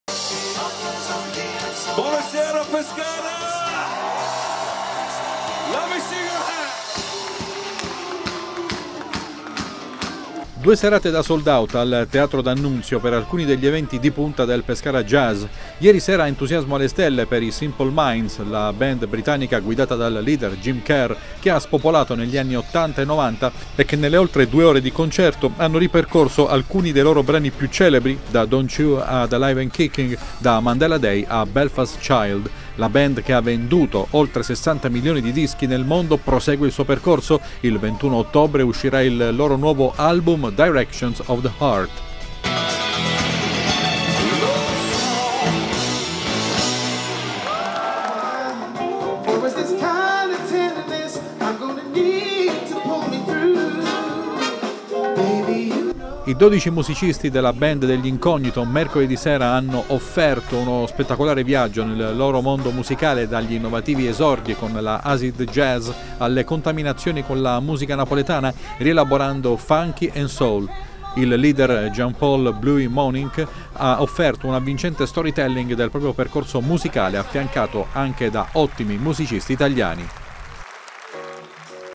Pescara Jazz. Entusiasmo alle stelle per i Simple Minds
Tutto esaurito ieri sera al Teatro D’Annunzio di Pescara per la data abruzzese dei Simple Minds che hanno ripercorso i successi più significativi della loro carriera. Molto successo anche mercoledì per gli Incognito